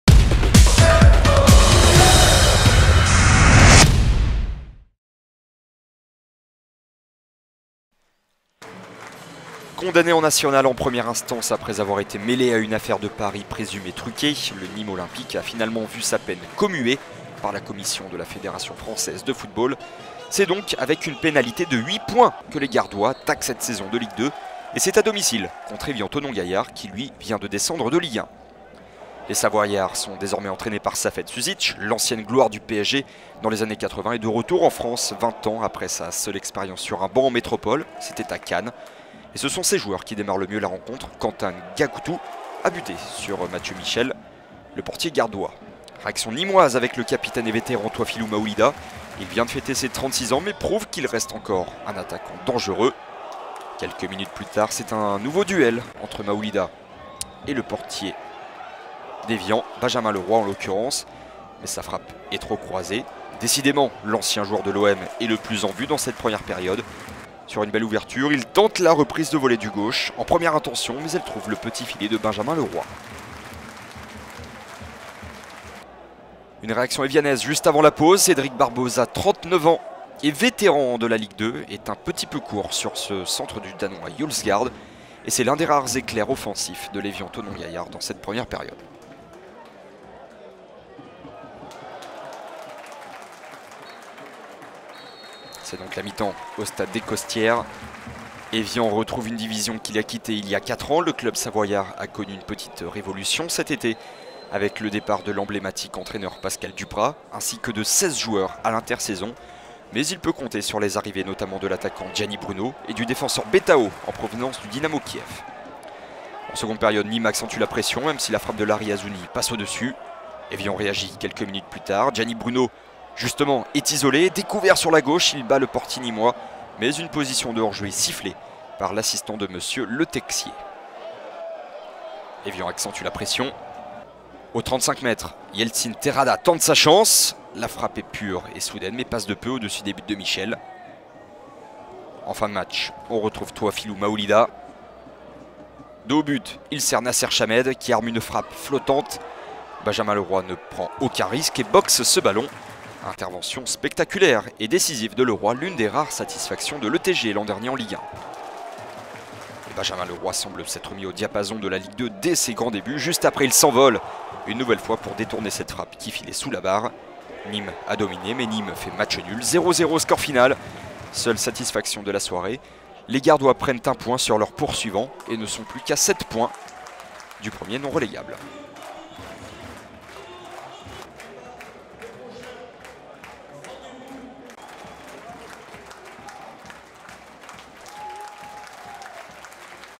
Nîmes Olympique - Evian TG FC (0-0) - Résumé - (NIMES - EVIAN) / 2015-16
Revivez les meilleurs moments de Nîmes Olympique - Evian TG FC (0-0) en vidéo.

Ligue 2 - Saison 2015/2016 - 1ère journée

Stade des Costières - vendredi 31 juillet 2015

Composition Nîmes Olympique : 1 - Mathieu MICHEL / 2 - Jérémy CORDOVAL / 3 - Romain ELIE / 5 - Larry AZOUNI / 8 - Toifilou MAOULIDA (c) / 10 - Anthony KOURA / 15 - Rémy SERGIO / 17 - Nasser CHAMED / 18 - Theo VALLS / 23 - Anthony BRIANCON / 29 - Fabien BARRILLON

Composition Evian TG FC : 3 - BETAO / 6 - Jesper JUELSGAARD / 7 - Quentin NGAKOUTOU / 8 - Yeltsin TEJEDA / 9 - Gianni BRUNO / 10 - Kévin HOGGAS / 14 - Cédric BARBOSA / 24 - Olivier SORLIN (c) / 27 - Clarck NSIKULU / 33 - Fabien CENTONZE / 40 - Benjamin LEROY